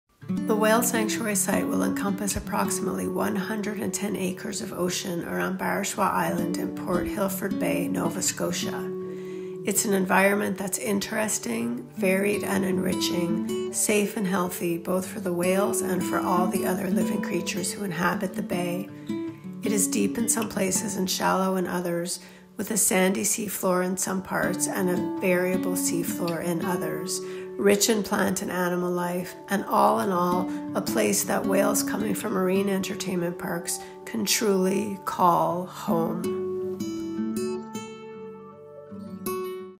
The Whale Sanctuary Site in Port Hilford, Nova Scotia
It is just over a year since the Whale Sanctuary Project announced that Port Hilford Bay in Nova Scotia would be the location of a gold-standard sanctuary that will provide for captive whales to be retired and given a new life.

And now, after a year of site development work, we’re delighted to have reached the next important milestone: deciding the specific placement of the sanctuary within this large bay.

There have been two main elements to reaching this next stage. The first involved several months of gathering environmental data to understand as much as possible about every aspect of the bay and its surrounding land.

That’s because the 110 acres that will be the whales’ home need to be an environment that’s interesting, varied and enriching, and one that’s safe and healthy both for them and for all the other living creatures who inhabit the bay. For example, the sanctuary area should be deep in some places and shallow in others; with a seafloor that’s sandy in some parts and a variable sea floor in others – rich in plant and animal life and basically a place the whales can truly call home.

To understand all of this, we’ve been documenting water depths around the bay, along with the nature of the seafloor below, and testing the water chemistry, the salinity, the temperature gradients, water flow transfer rates and freshwater egress into the bay. Most recently, we completed the hydrodynamic model, which tells us what we need to know about currents, tides, waves and surges so that we can position the sanctuary very precisely and place the nets accordingly.

The second aspect of site development has involved understanding how the sanctuary can work most successfully with all the people who have an interest in the bay and a relationship to it. These include the local communities of Port Hilford, Wine Harbour, and Sherbrooke and the district of St Mary’s; the people who live and work on the bay; the Mi’kmaq Nation; the federal, provincial and local officials who are charged with issuing the necessary permits; and with other interested people and organizations of all kinds throughout Nova Scotia.

Accomplishing all of this has been something of a challenge in that right on the heels of our announcing Port Hilford as the location for the sanctuary, the COVID-19 virus all but shut down travel between the United States and Canada (and indeed between Nova Scotia and the rest of Canada). That made the next stage of our work a lot more complicated. But with lots of teleconferencing, along with help and support from the local community, we were able to press ahead.

Finally, just two weeks ago, it all came together. And at a community meeting in Sherbrooke, Nova Scotia, we were delighted to announce that the sanctuary will be located in 110 acres of ocean around the wharf near the mouth of Port Hilford Bay.

This location has the full support of the fishermen who moor their boats at the wharf and fish for lobsters in the waters nearby. They are truly rolling up their sleeves with us. And while we’re no longer surprised by that level of support, it is just terrific.

We have also reached an agreement in principle – subject to our having all the governmental permitting in place – to purchase more than 100 acres of prime coastline that will border the sanctuary waters and will provide exclusive use of the wharf. By having that much of the coast included in the sanctuary site, we have a truly enriching area for the whales. It has the deepest water – 18 meters of depth off Barachois Island – and shallows as well. There’s a lot of variety in the substrate and the coast, which is exactly what’s needed for a whale sanctuary.

Being able to settle on a specific location within Port Hilford Bay leads us into the next stage of permitting with the Nova Scotia Department of Lands and Forestry.

It also means we must immediately be even more active in raising the funds necessary to build the sanctuary itself, and we look forward to describing this in more detail. And with your continuing help, we look forward to being able to welcome the first whales by the end of next year.

Thank you, as always, for making this such a unique opportunity for the whales. Together, we can give them a new life that makes up for so much of what they have endured.